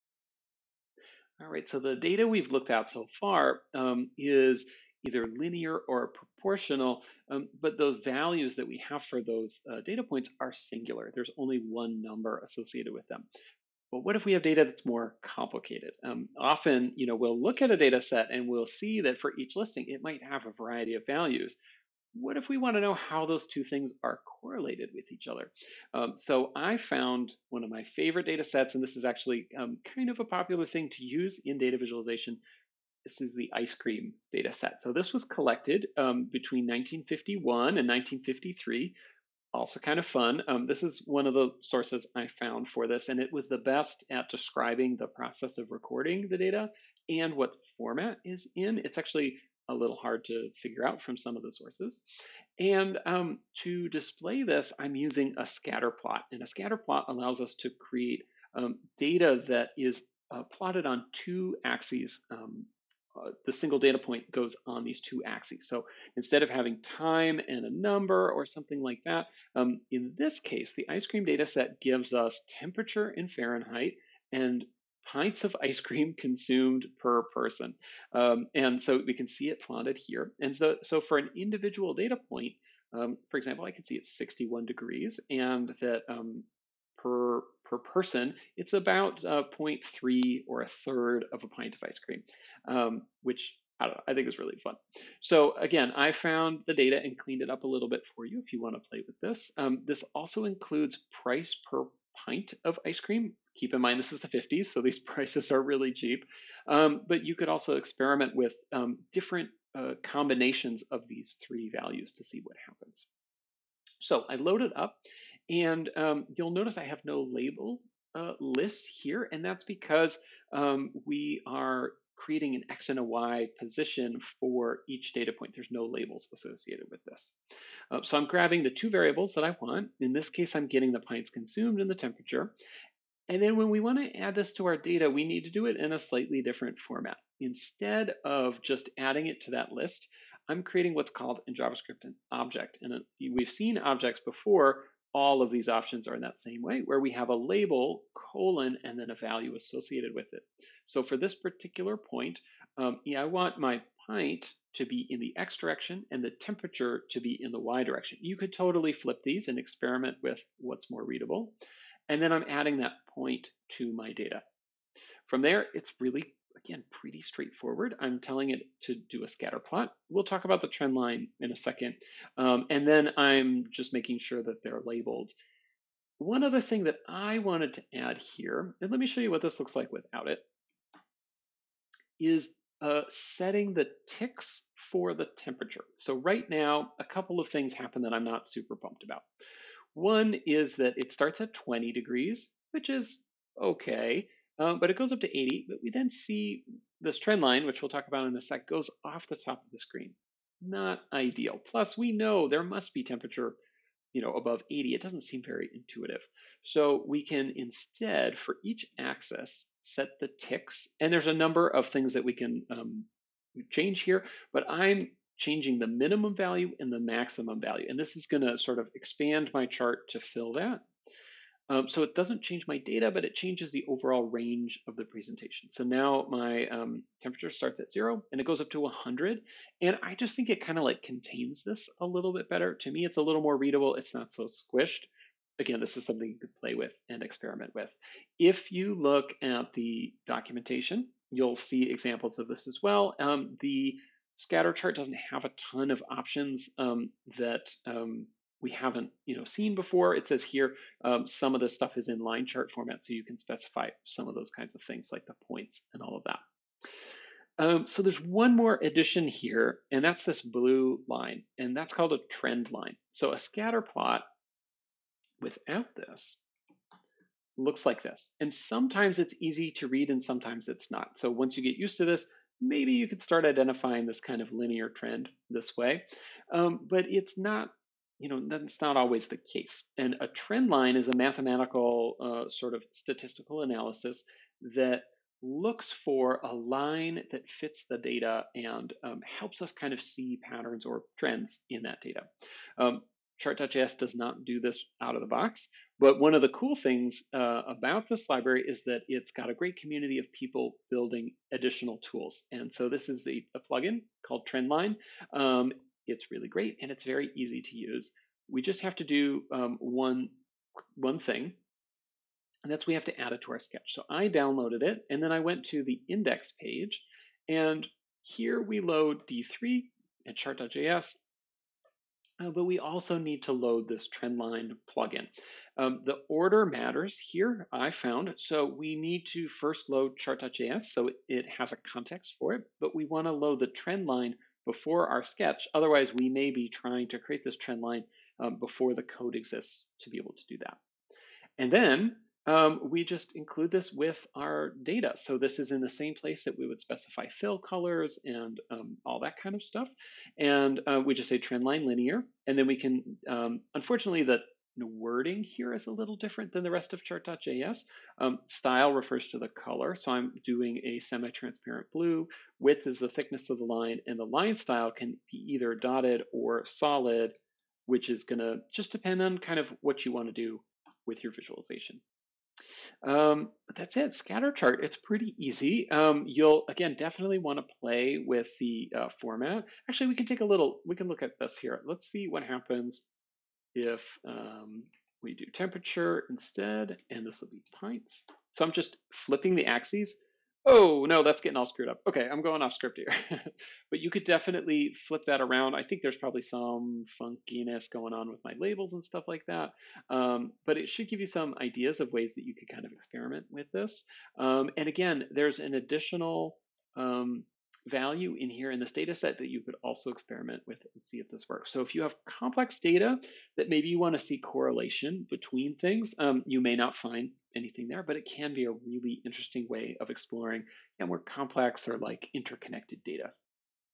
Scatter Chart – Other Visualization Formats/Designing With Data
In our previous examples, our data has been singular: one number for each item. However, data often has multiple elements that may be inter-related in some way. The scatter plot is a great way to see that!

Instead of a single value for each data point, we can specify two – one in the X direction and one in the Y. In this example, we use ice cream sales data from the early 1950s – a classic of data visualization.

CHALLENGES
❓ There is an additional column in the data for price per
   pint. Try using that instead!